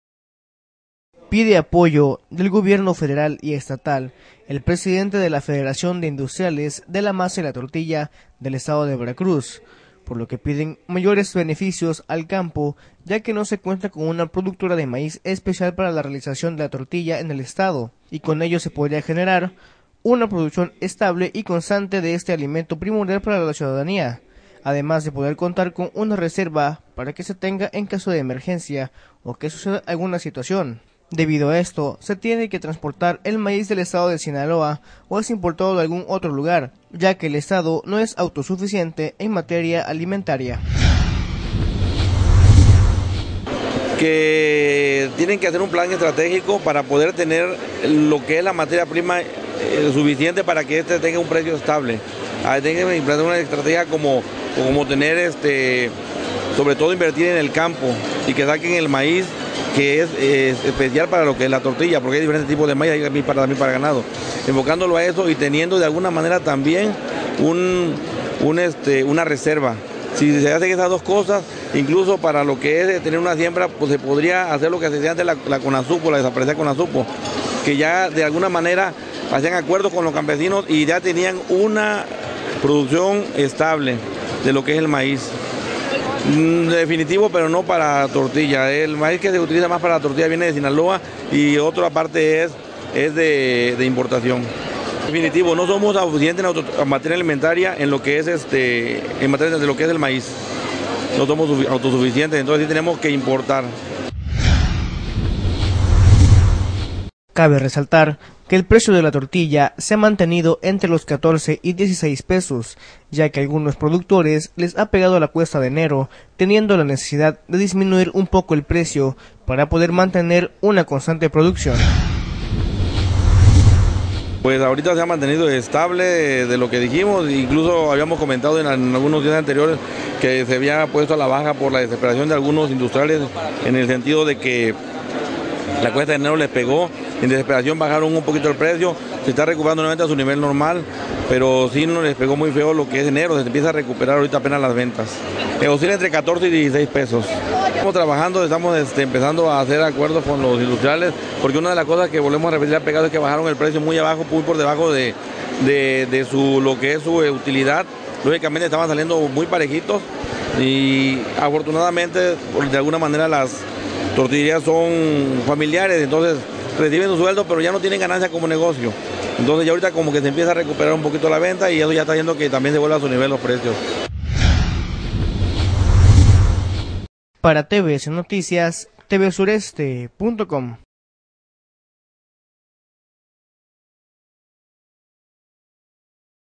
TVS Noticias.- Insuficiente reserva de Maíz en Veracruz para producción de Tortillas
- Insuficiente reserva de Maíz en Veracruz para producción de Tortillas